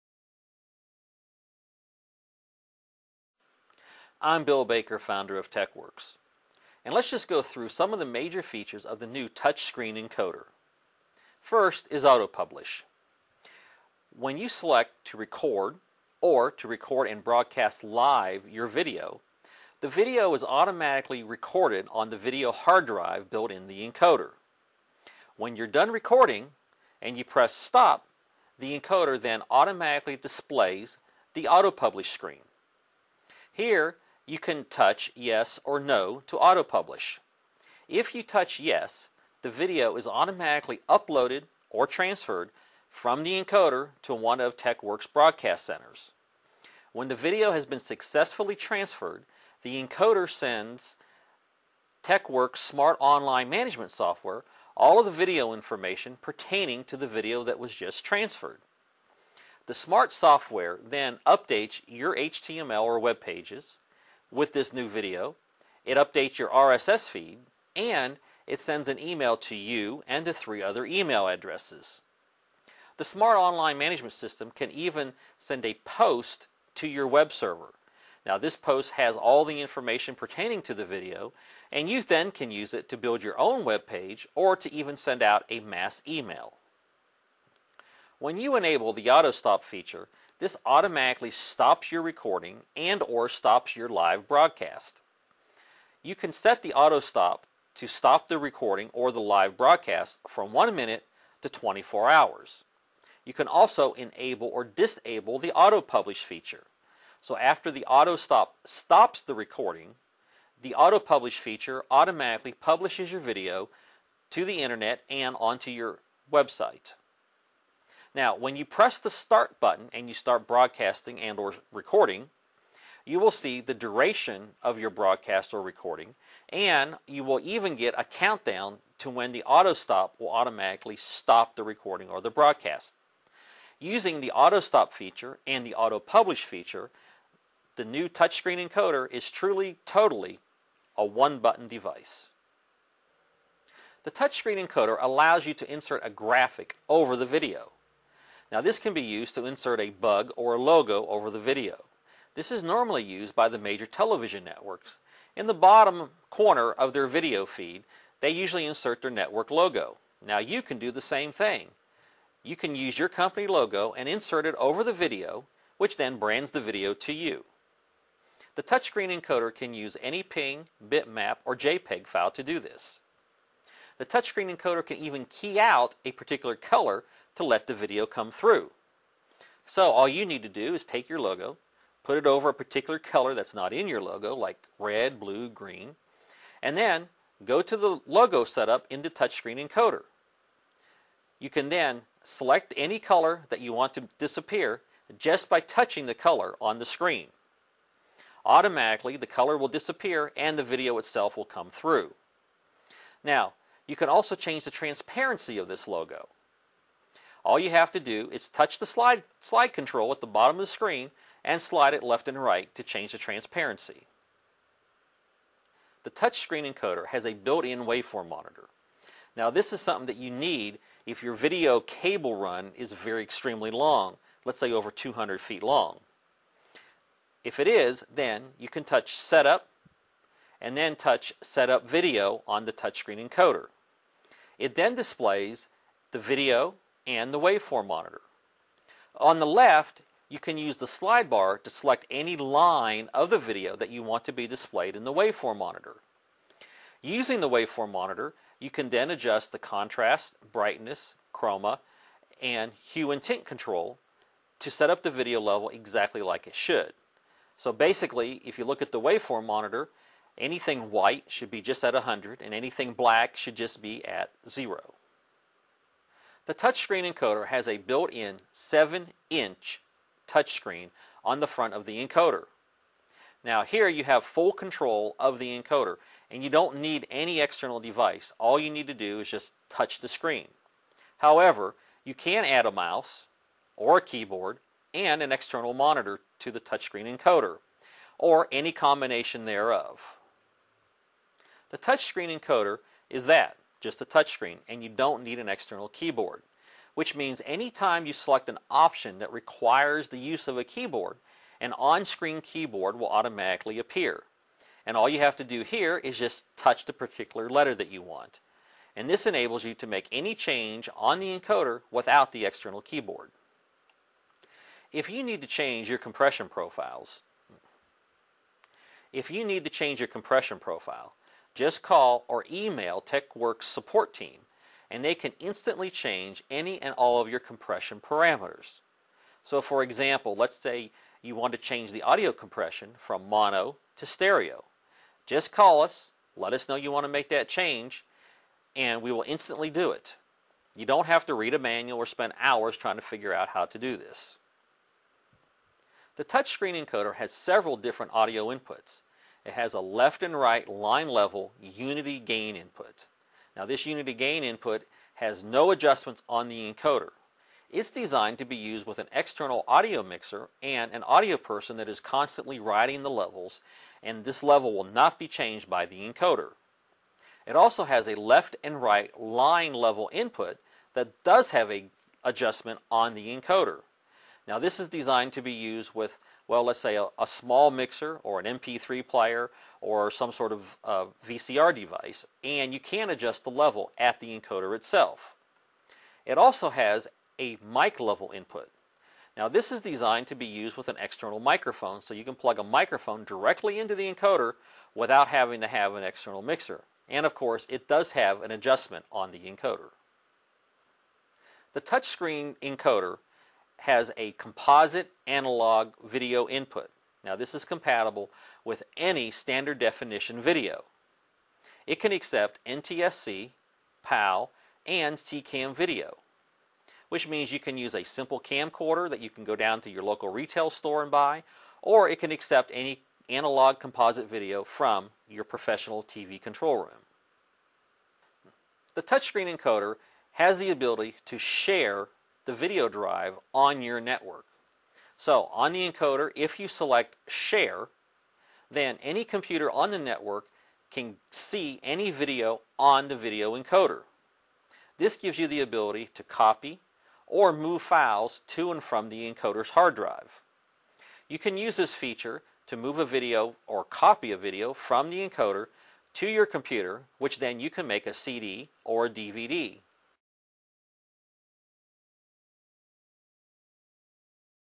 EZ Caster - Features Overview
Techwerks EZCaster allows anyone to shoot, store, and host video content and then steam it over the internet! This small box packs a ton of features, watch the video for more details.